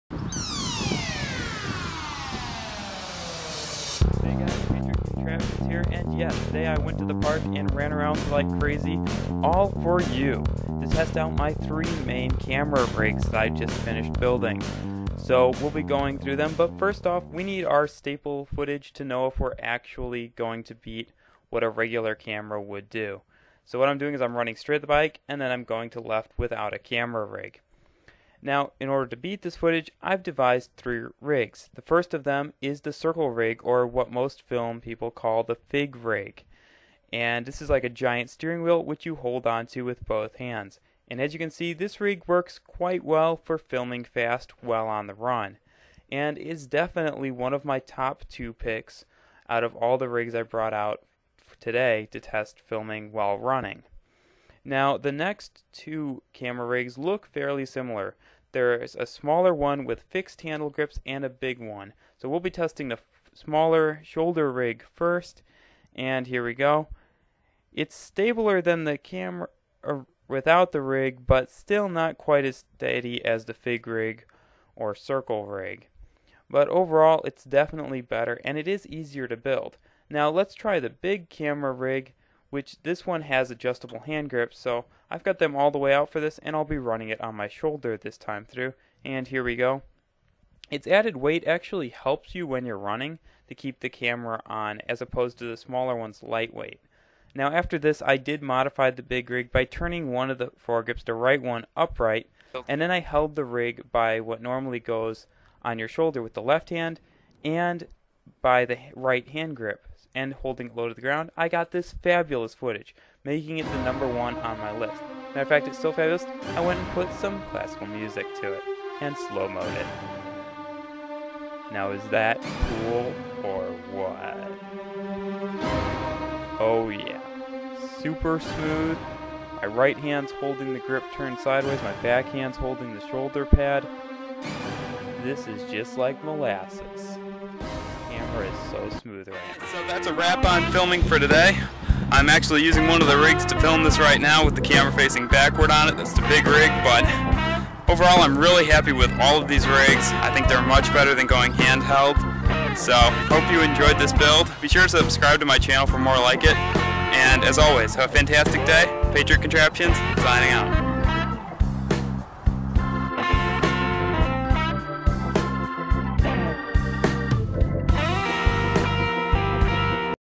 Filming Cargo Bike for Fun- Homemade No Budget Film Rigs
Today I test out Three diffrent camera rigs I have made a Fig Rig(Circle Rig), Little Shoulder rig(steady cam), and Big Shoulder Rig(Steady cam).  Each rig offers different pros and cons but do they really help you film while running full speed as if filming a action scene?  This is also inadvertently a sad video as on the rid back I crashed the cargo bike and decided the repair to the bent and broken frame would be too much work.  Also I was under pressure to clear out some of my my bike builds from the house.

PS Use All Information at Your Own Risk!!!